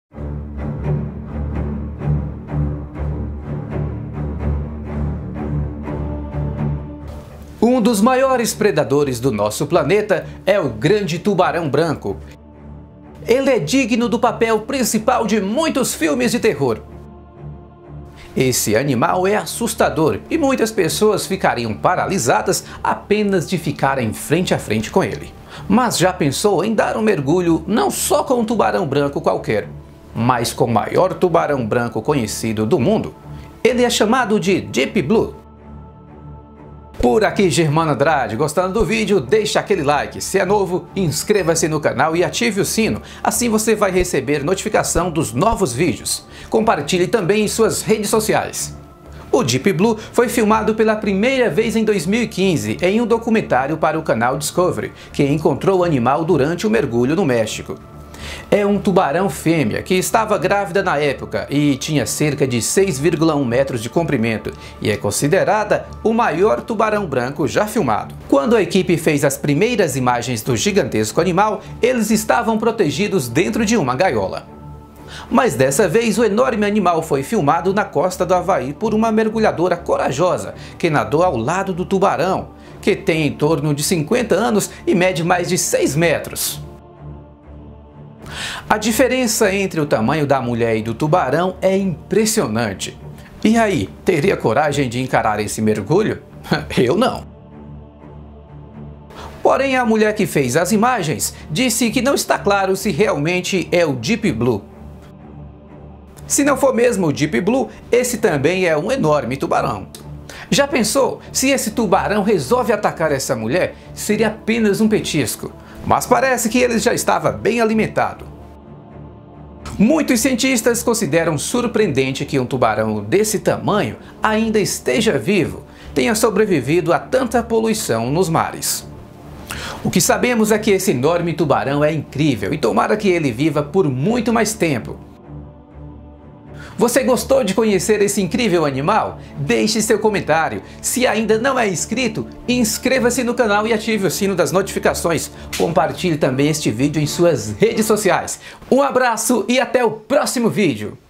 MAIOR TUBARÃO BRANCO DO MUNDO DEEP BLUE
Veja essas imagens incríveis de uma mulher mergulhando com tubarão gigante chamado de deep blue que é o maior tubarão branco do mundo já filmado.

O Deep blue é o maior tubarão branco do mundo já encontrado e essa é sua segunda aparição.